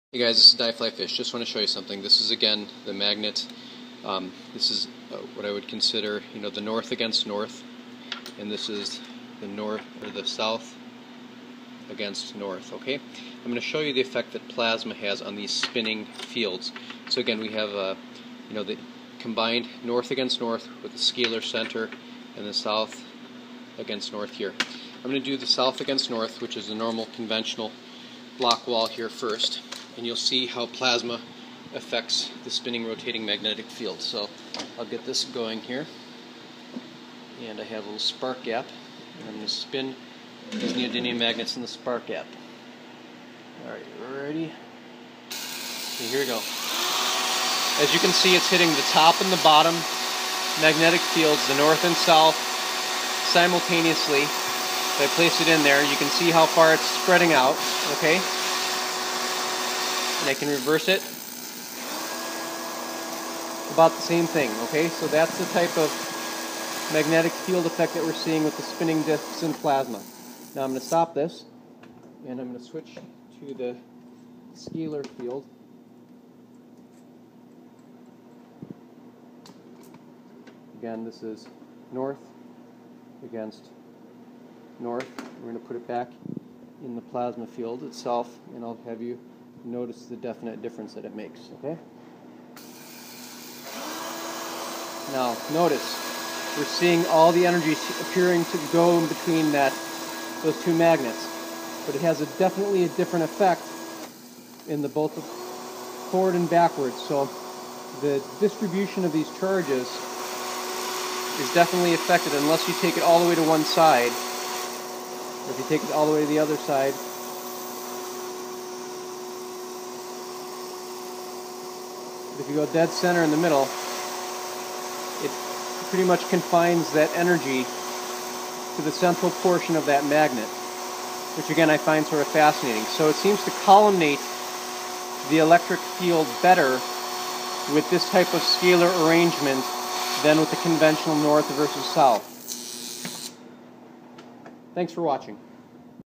Rotating Mag Fields In Plasma N/S vs S/S
This Video is to show how Rotating Magnetic fields interact within a OPEN plasma, i.e. spark gap. The source of the spark gap is a Stout Bar Circuit which is NOT shorted thus conventional plasma is produced. There is a MARKED difference when a conventional N/S magnetic disc is spinning within the plasma vs a S/S magnetic field. In the N/S arrangement, the plasma is pulled to either face of the spinning discs, and the plasma spreads out dramatically. When a S/S disc magnet arrangement is spinning within the open plasma, the plasma appears to be very collimated to the center of the spinning magnets. This arrangement seems a logical way to have magnets arranged for a FARADAY type disc in so far as the energy seems to be drawn to the center instead of the periphery of the magnetic disc. Perhaps an adjunctive next step would be to have two discs being N/N and S/S counter rotating in a plasma. Which brings up the point, could a spark gap plasma be used instead of a Brush? Can a faraday disc produce significant current while the spark gap provides massive voltage. ?
All thoughts welcome
Very Best Regards,
Diveflyfish